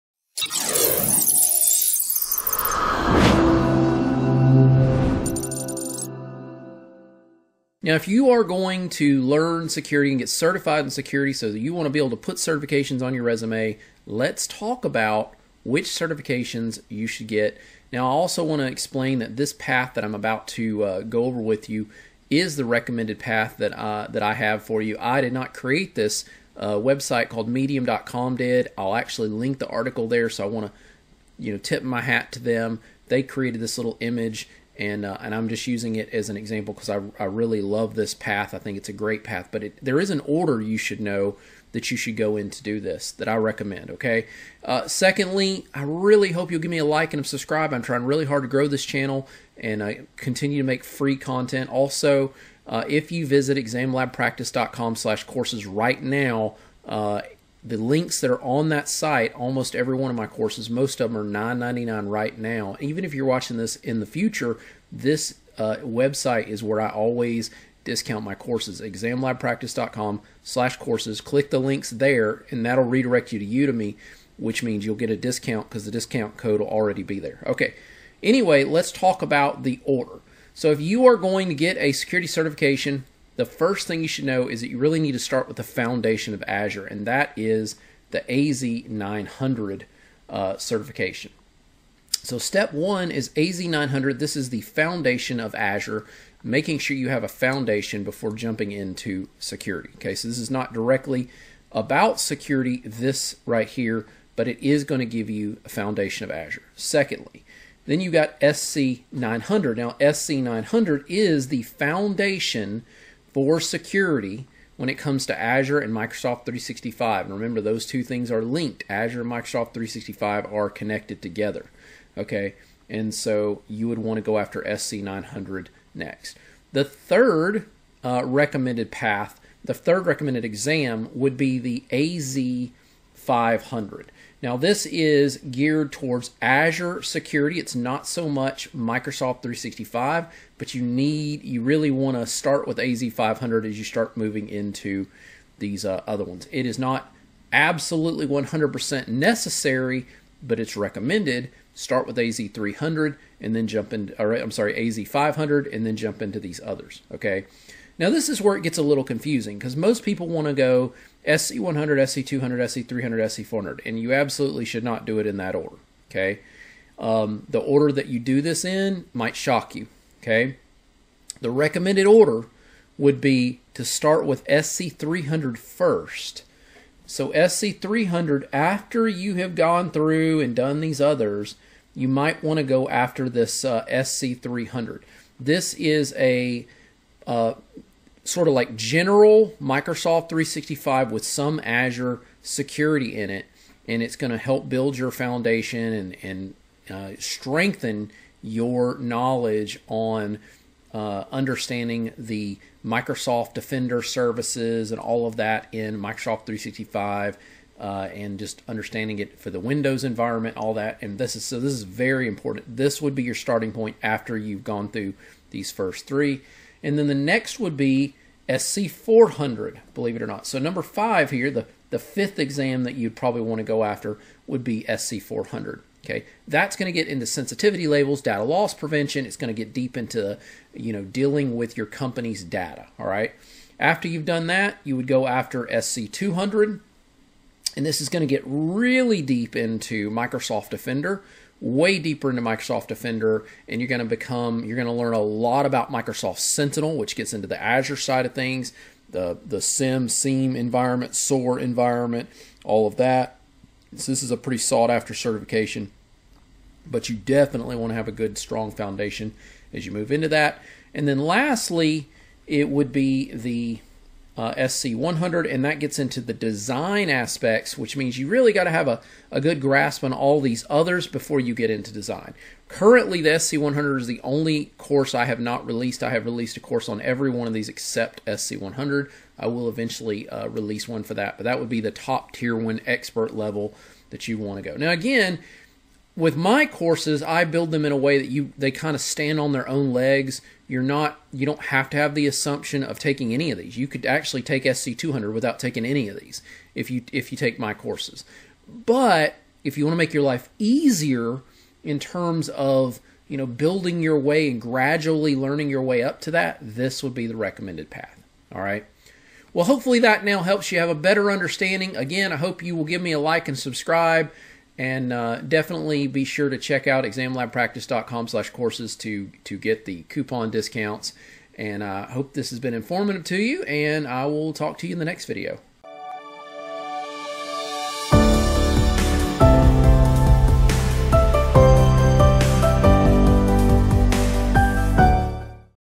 Exam path for Microsoft Security certification
I've noticed lots of confusion on what path someone should take if they are looking to get certified in Microsoft Security. In this video, I'm going to walk you through my recommendation on this.